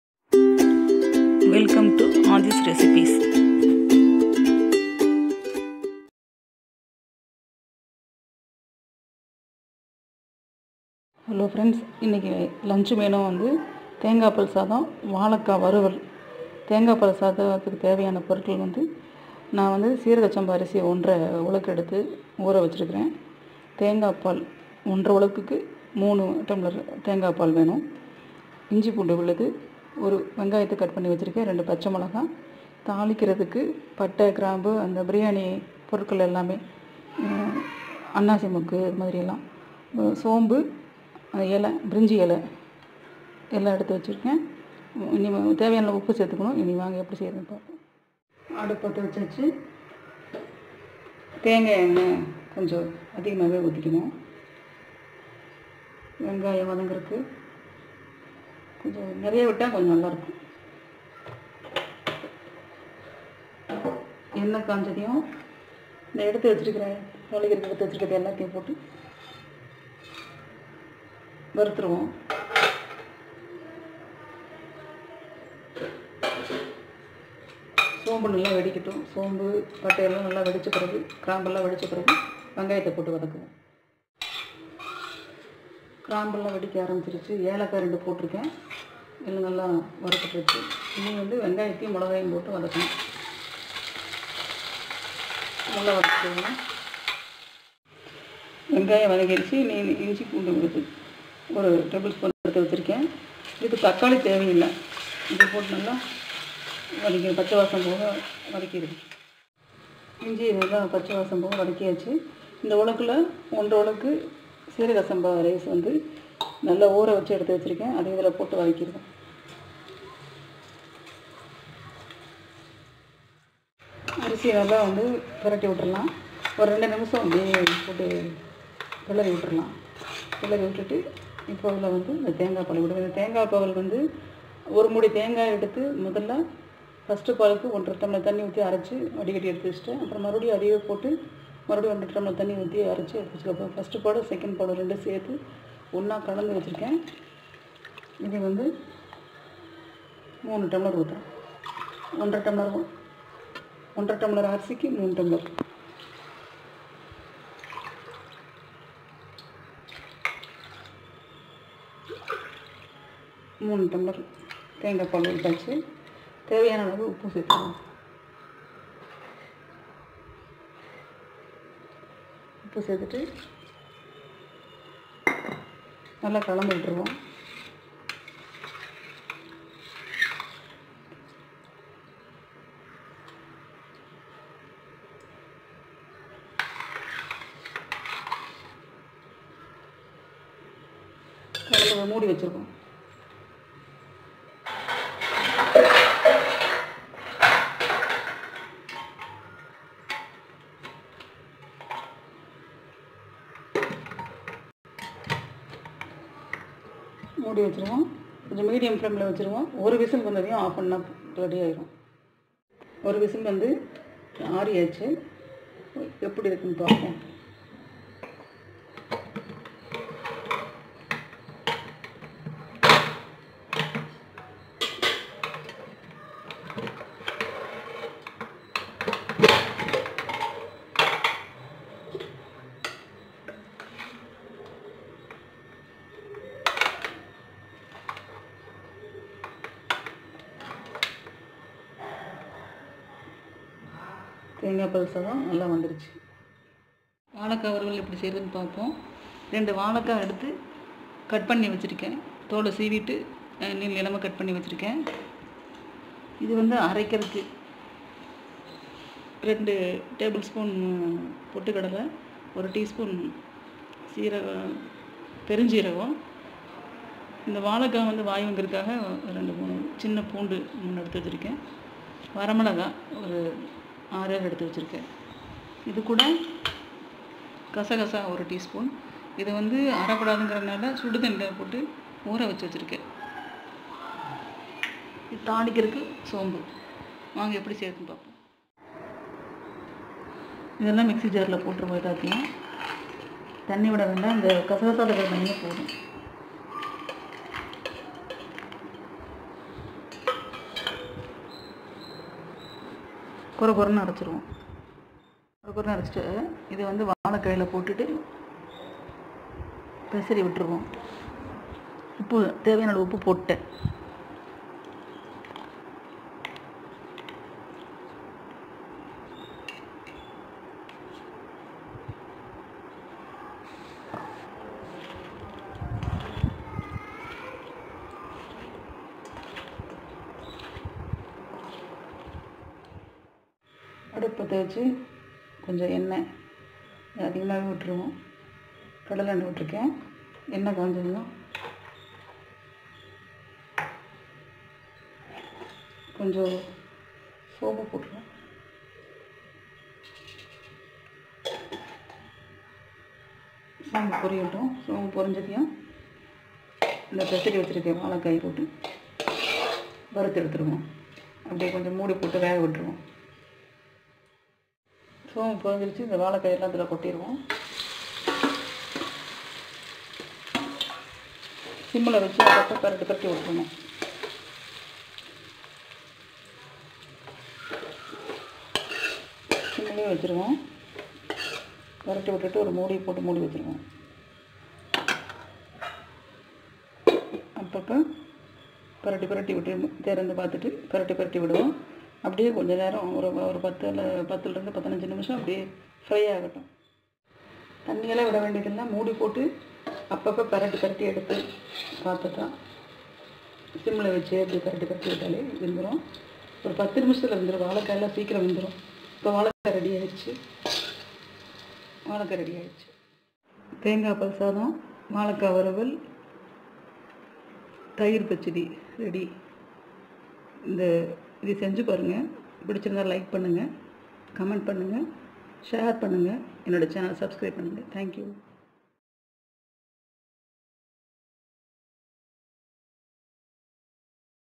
Coconut milk rice with vazhakkai varuval- lunch menu-2 தேங்காய்ப்பால் சாதத்துடன் வாழைக்காய் வறுவல்
Ingredients vazhakkai varuval-

Vazhakkai-2
For masala

Bengal gram-2 tablespoon-
Red chilli-7 nos.
Anis(  சோம்பு)-1 teaspoon
கசகசா- 1 teaspoon (poppy)
Garlic -2 nos.
For seasoning - சோம்பு 1teaspoon
Oil for frying vazhakkai & seasoning

Coconut milk biryani
crated coconut- 2 cups for taking coconut milk
Onion-1
Green chilli-2
For seasoning-பட்டை, கிராம்பு, ஏலக்காய், அன்னாசி மொக்கு, கல்பாசி, பிரிஞ்சி இலை.